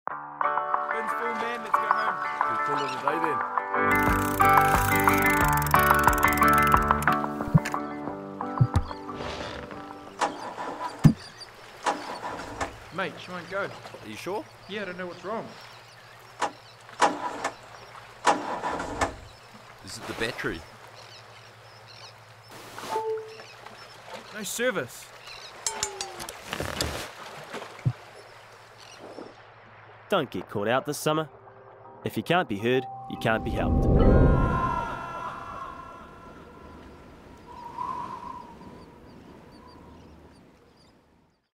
When at sea - Waterproof communications
If you can’t be heard, you can’t be helped! Always carry at least two waterproof ways to call for help.